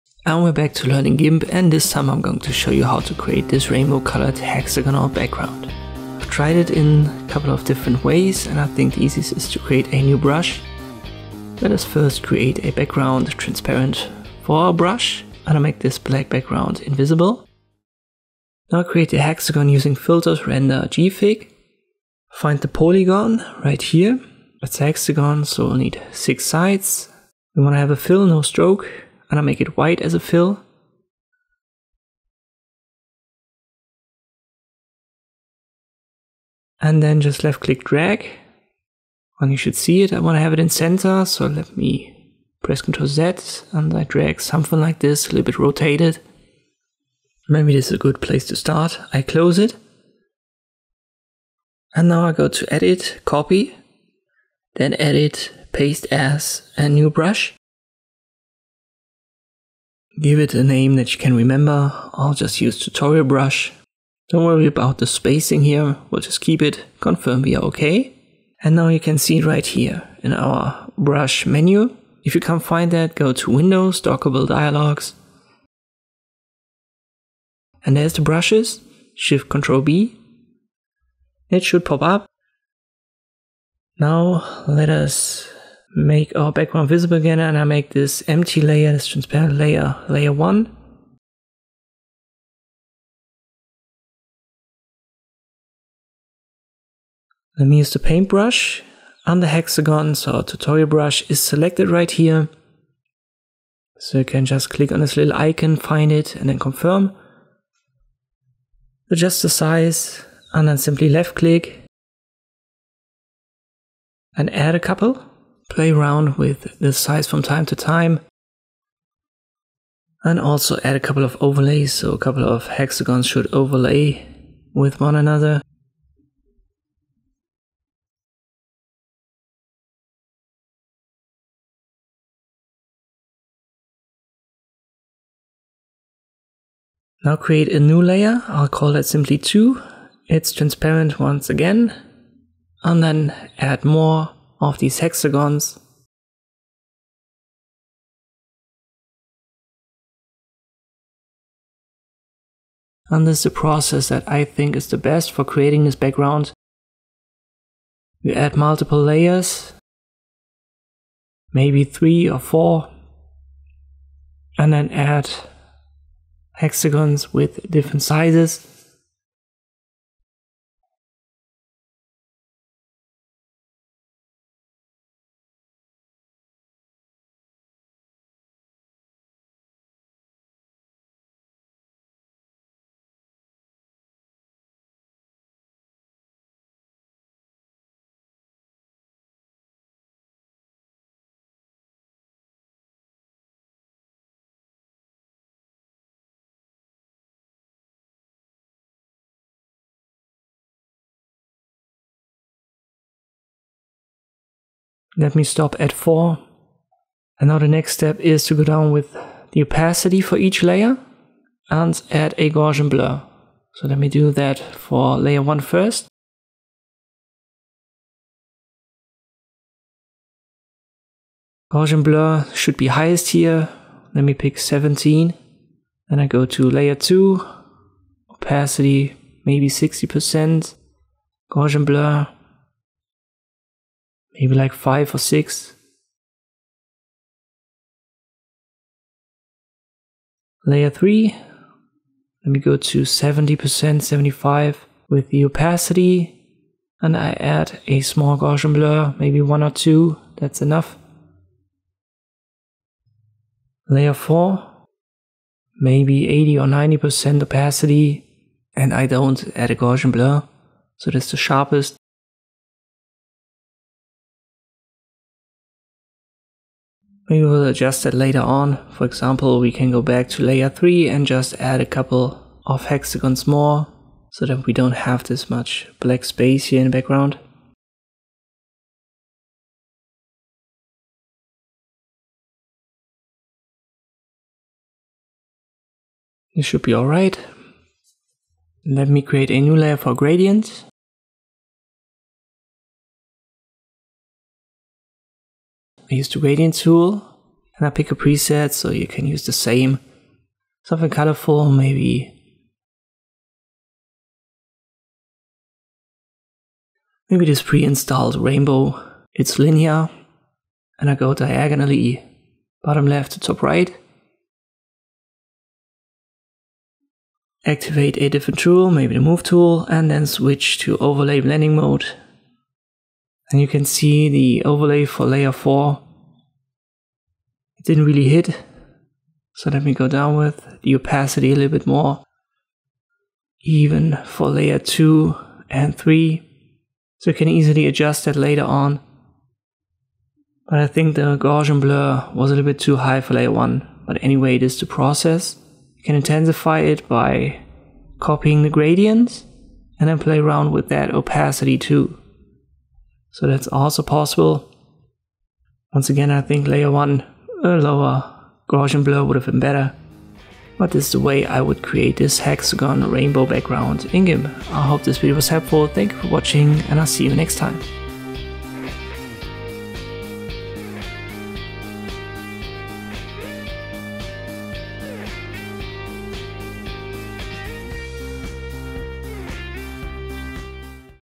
How to Create a Hexagon Background in Gimp
In this tutorial I'll show you how to create a hexagon background in Gimp.